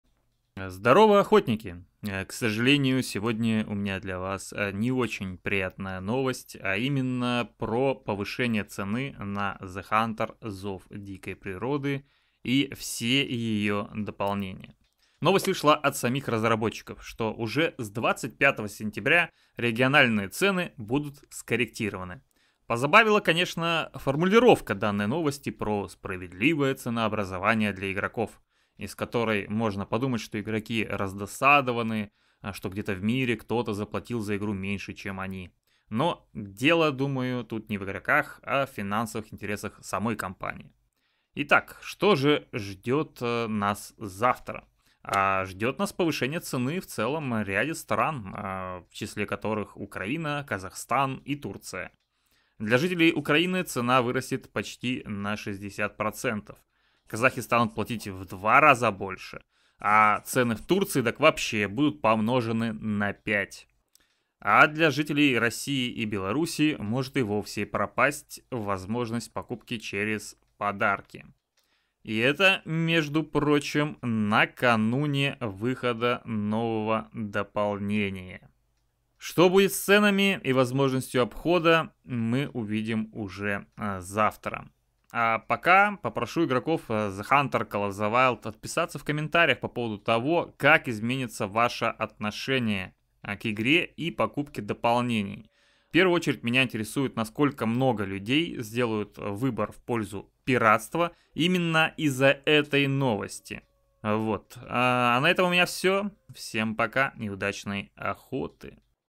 Рост цен на theHunter Call of the Wild
25 Сентября цена на игру theHunter Call of the Wild и все её дополнения будет повышена !
В каких именно странах и на сколько - в этом видео.

Тайминги, для вашего удобства:
0:00 - Вступление
0:14 - Новость о повышении цен
0:41 - В каких регионах вырастет цена
01:19 - Поделитесь своим мнением